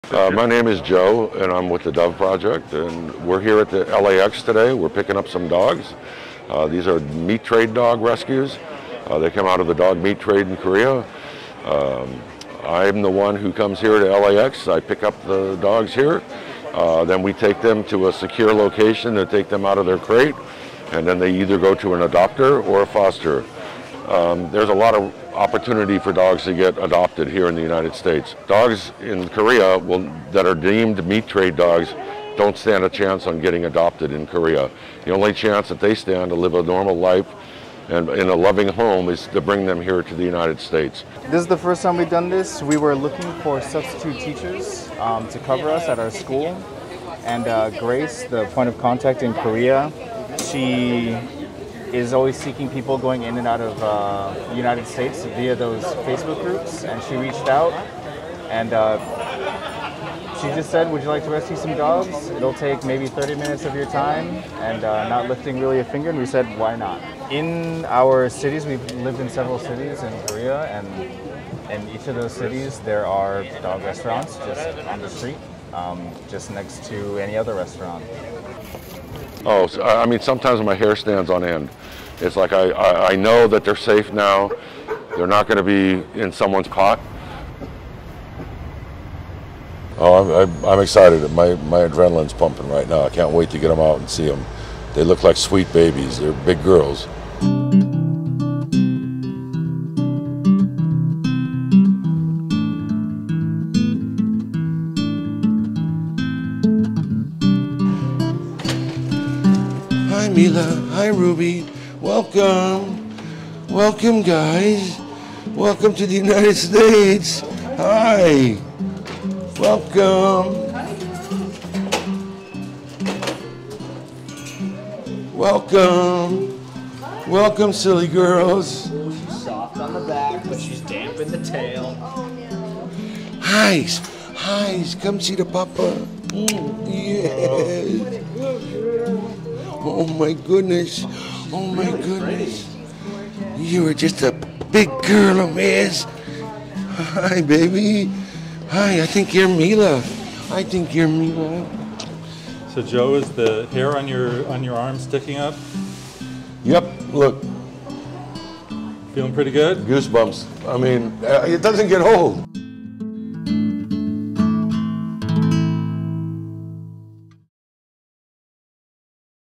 The Dog Rescuer
Meet Joe Bahm, who rescues dogs from dog meat restaurants in Korea. See him meet the new arrivals at LAX and get them ready for their new homes. For the Easy Reader of Los Angeles